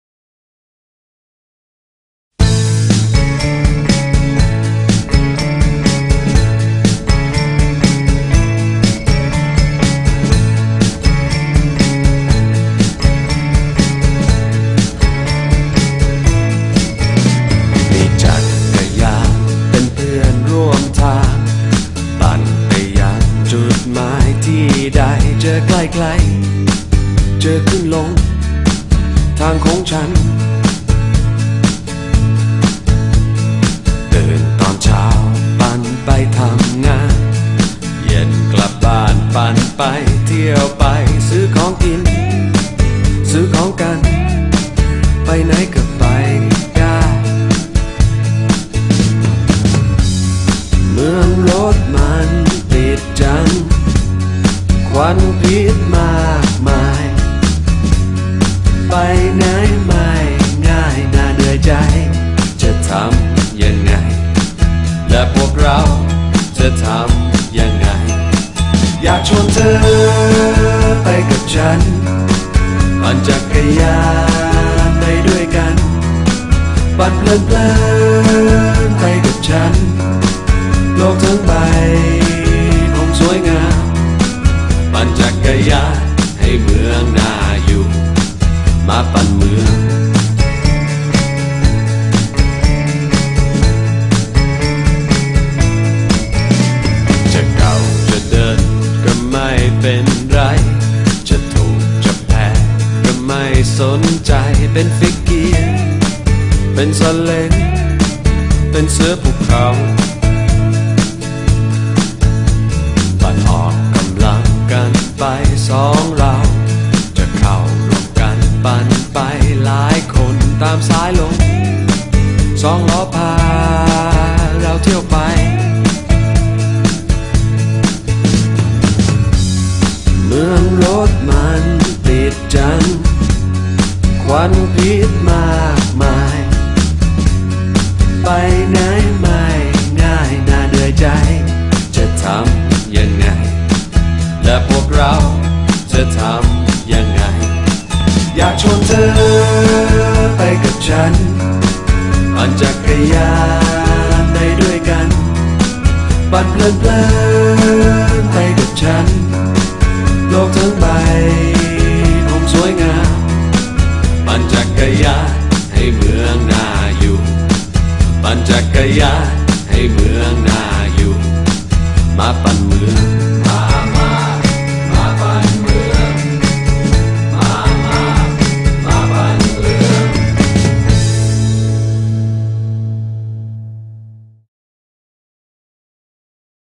มา..มา ปัน ปั่น ปั้น2
อีกครั้งกับกิจกรรมสบายๆสไตล์ INSouth | 18-19 ต.ค. 2555 กับ " ปัน ปั่น ปั้น 2 "
ระยะทาง From บ้าน INSouth To หาดตะโละสะมิแล ยะหริ่ง ปัตตานี
ตัวเลข สถานะ ไม่จำกัด กายพร้อม ใจพร้อม ลุย!!